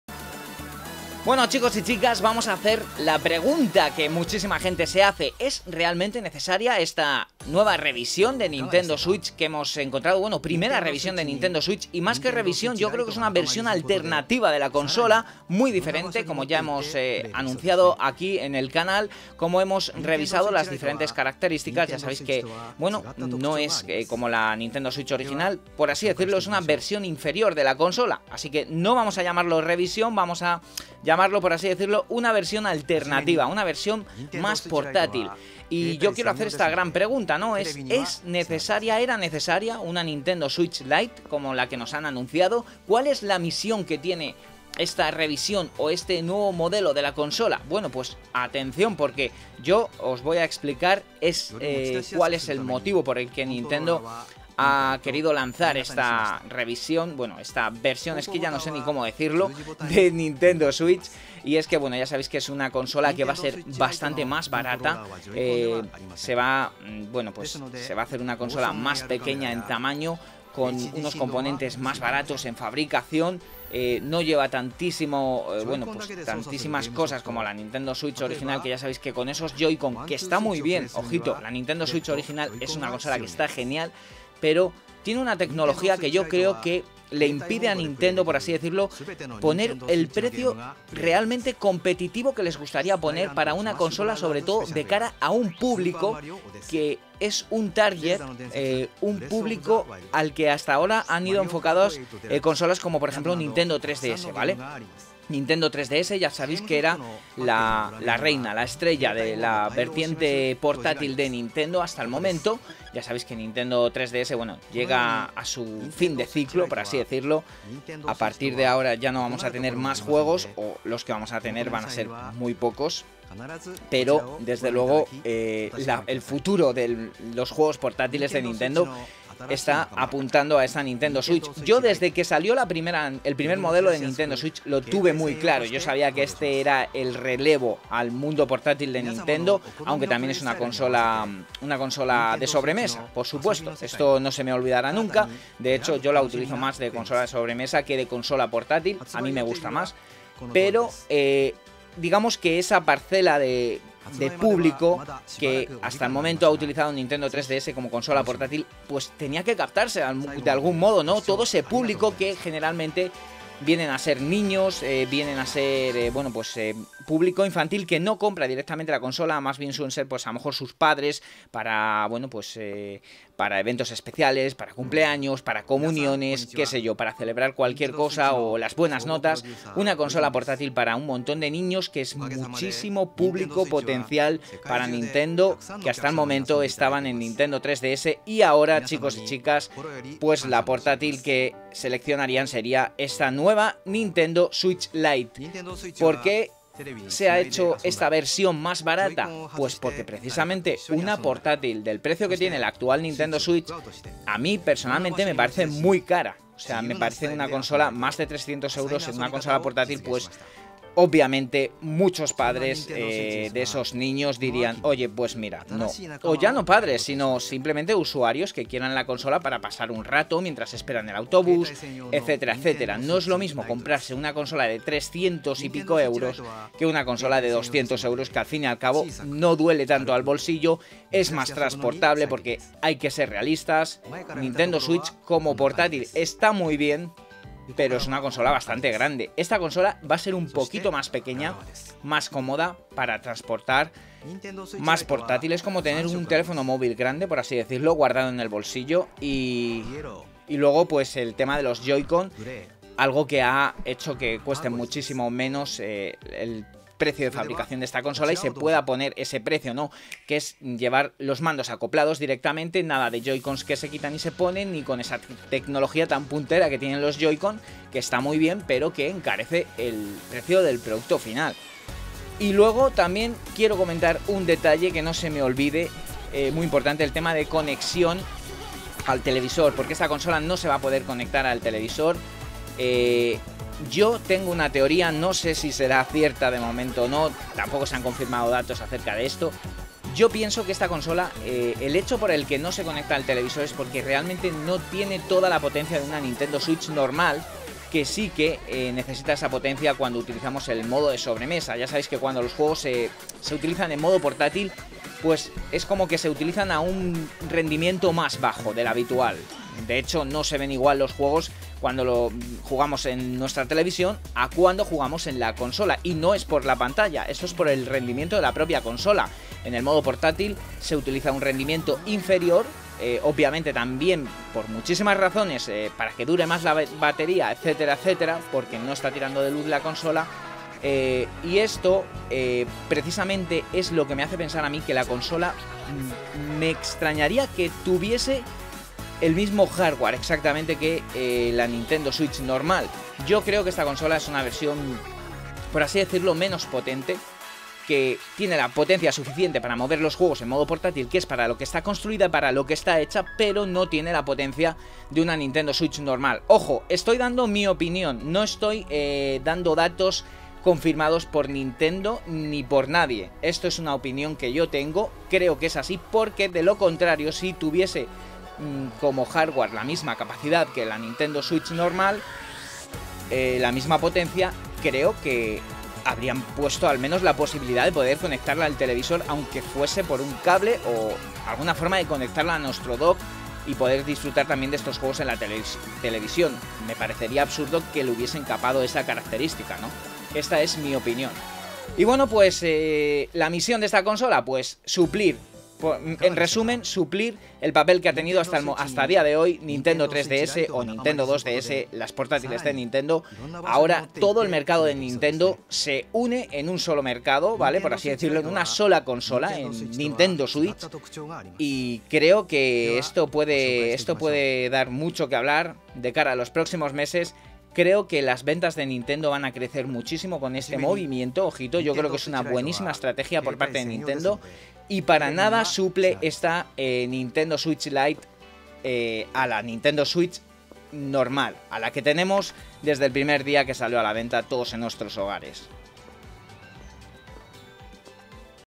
¿Era necesaria la Nintendo Switch Lite? ¿Cuál es la misión de esta revisión?
No te pierdas la información más importante del mundo de los videojuegos

Estate atento de las últimas noticias dándole a la campanita
Fuente: OPINIÓN PROPIA